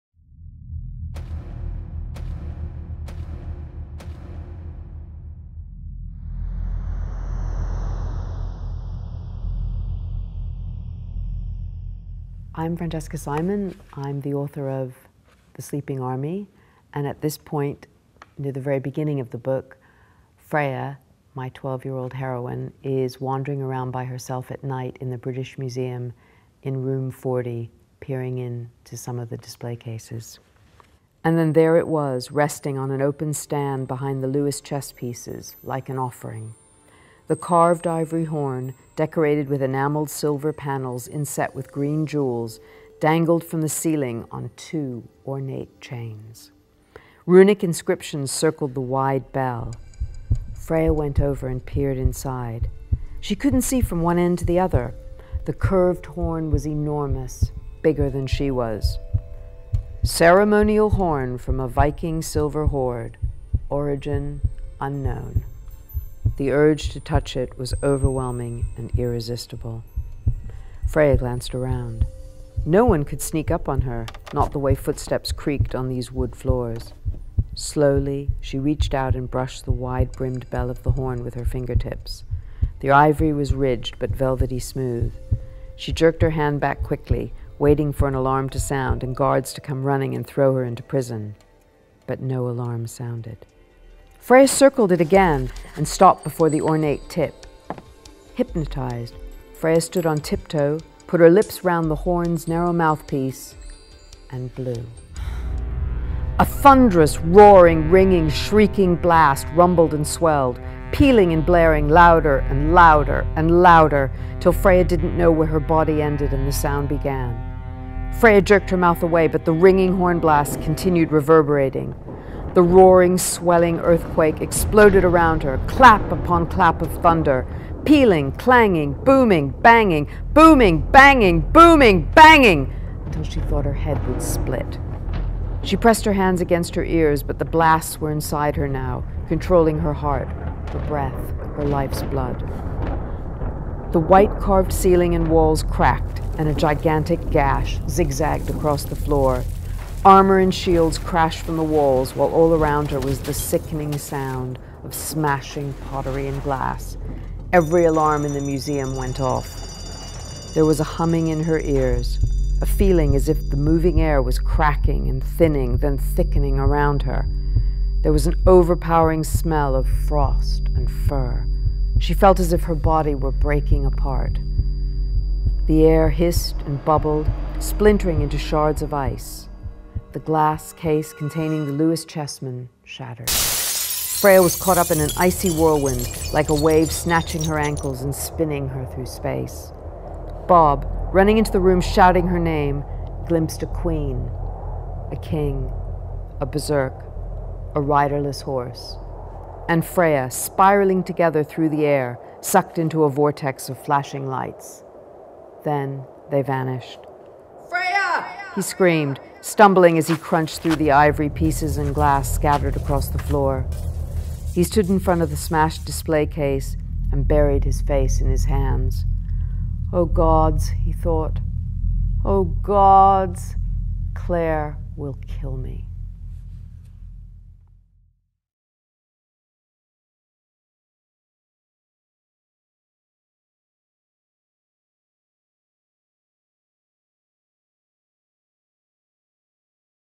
Francesca Simon reading The Sleeping Army
One of the bestselling children's authors in the world, the creator of the "Horrid Henry series", launches an exciting new novel for children aged 8+. Freya is an ordinary girl living in modern Britain, but with a twist: people still worship the Viking gods. She's caught in her parents' divorce, and shuttling between bickering adults is no fun. One evening, stuck with her dad on his night shift at the British Museum, she is drawn to the Lewis Chessmen and Heimdall's Horn. Unable to resist, she blows the horn, waking three chess pieces from their enchantment; the slaves Roskva and Alfi, and Snot the Berserk. They are all summoned to Asgard, land of the Viking gods, and told they must go on a perilous journey to restore the gods to youth. If Freya refuses she will be turned into an ivory chess piece but, if she accepts her destiny and fails, the same terrible fate awaits her.

Buy The Sleeping Army by Francesca Simon on Waterstones.com or at your local Waterstone's